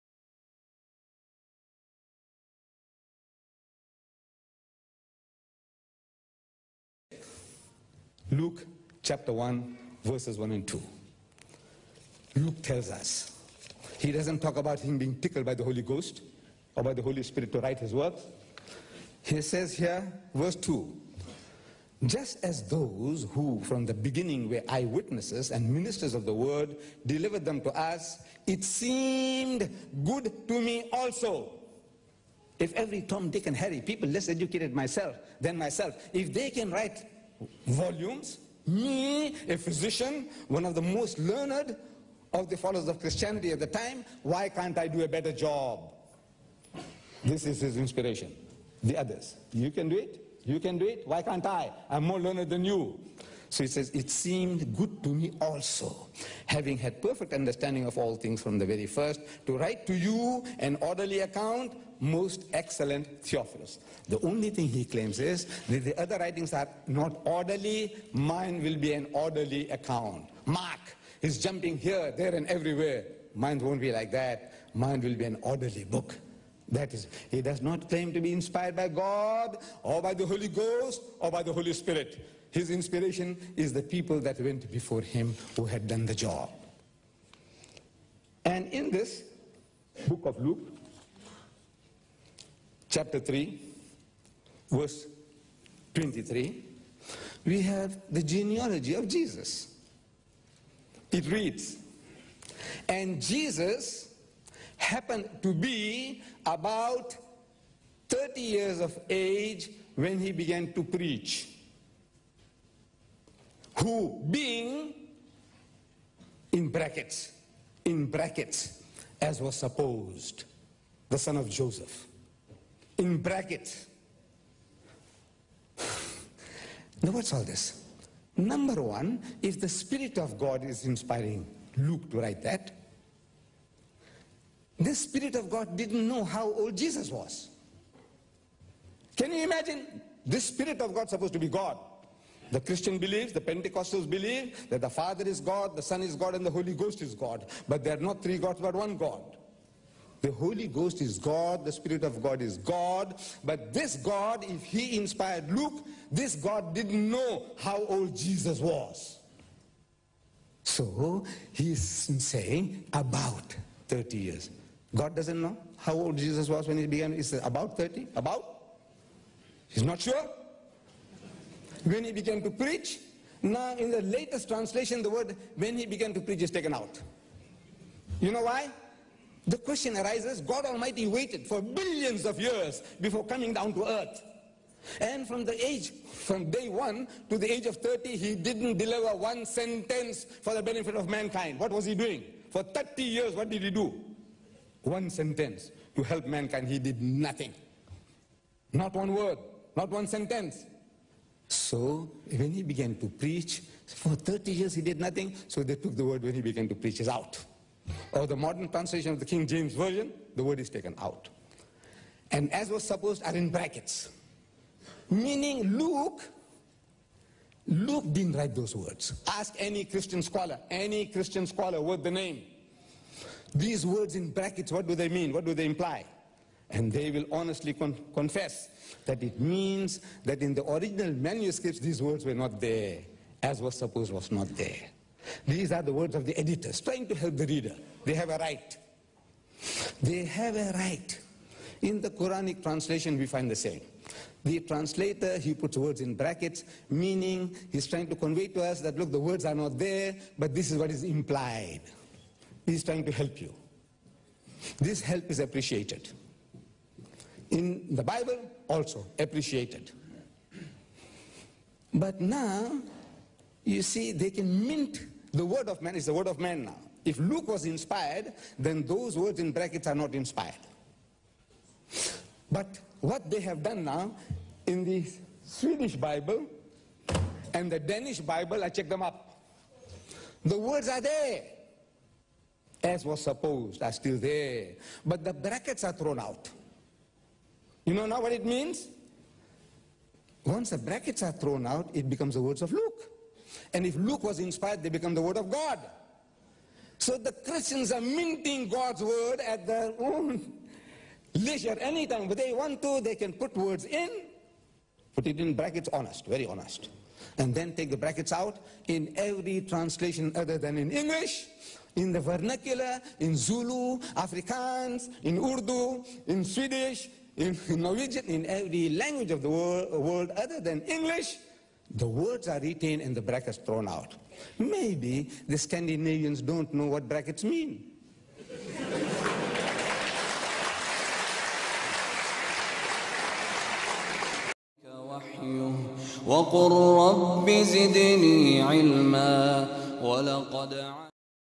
Ahmed Deedat - God Himself don't know how old was Jesus(pbuh)
Epic Debate happened between Ahmed Deedat and Pastor Stanley on the topic "Is Bible God's Word" where Alhamdullilah, Ahmed Deedat turned the tables and showed some heinous  errors in the so called "Word of God - Bible".